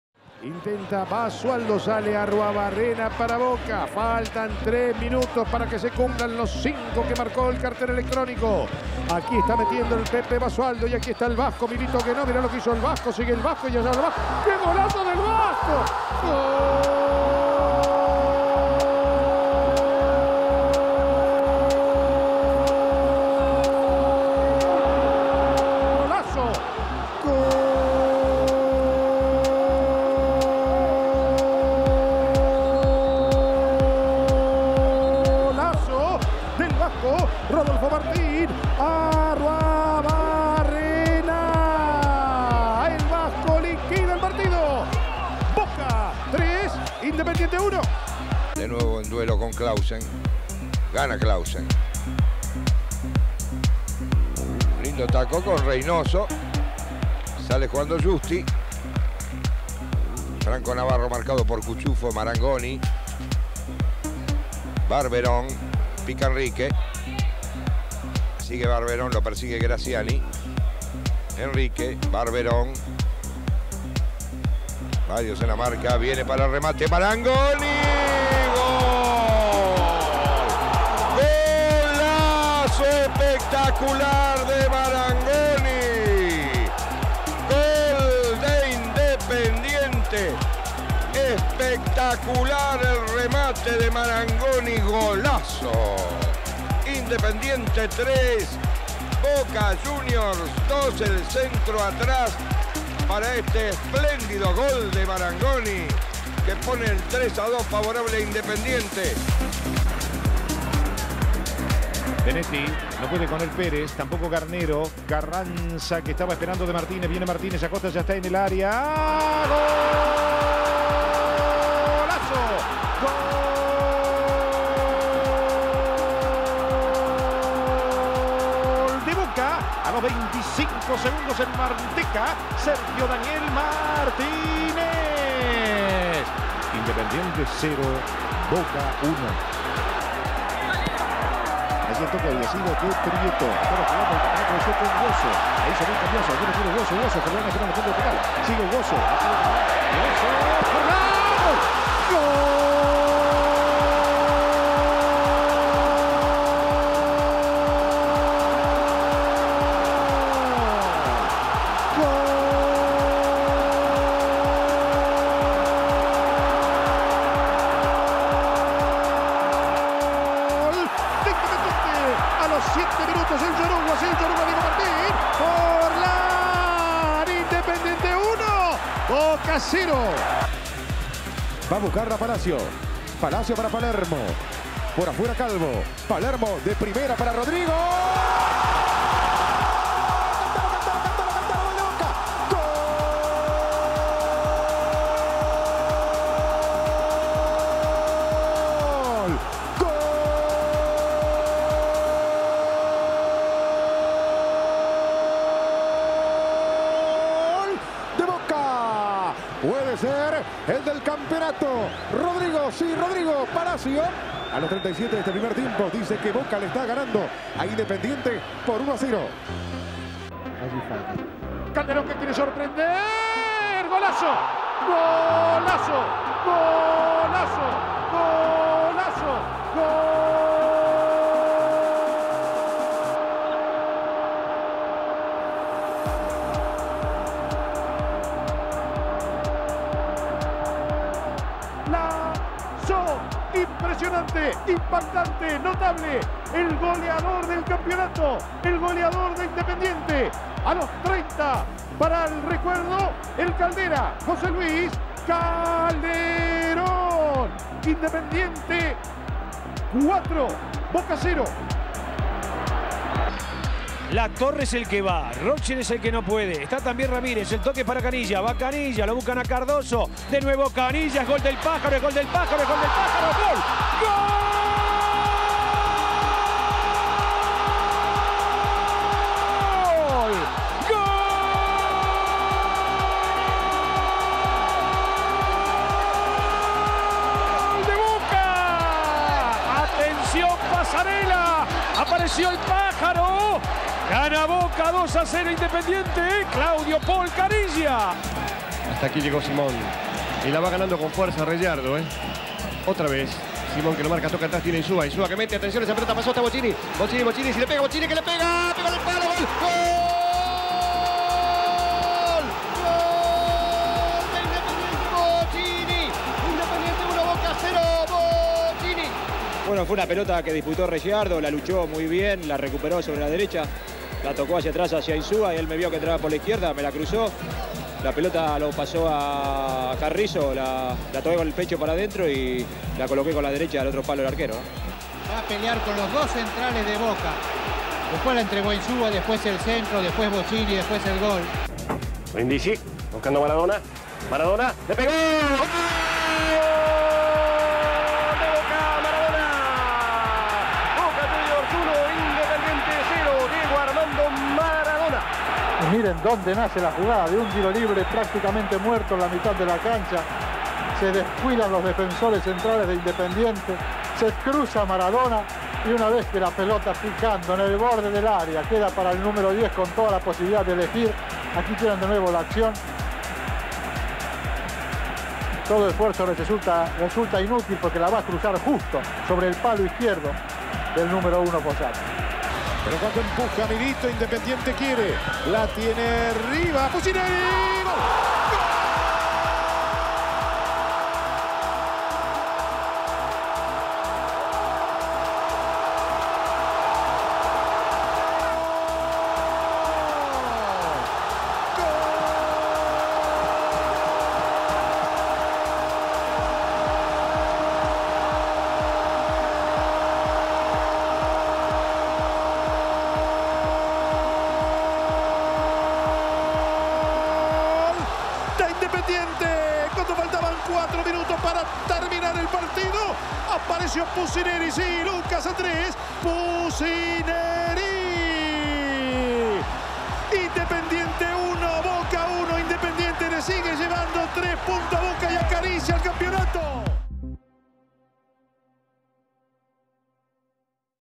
🎥 10 GOLES DE INDEPENDIENTE Y BOCA EN AVELLANEDA
Calentando la previa del Independiente vs Boca, repasamos 10 golazos de en Avellaneda 🔴⚪️🔴 ⚽️ 🔵🟡🔵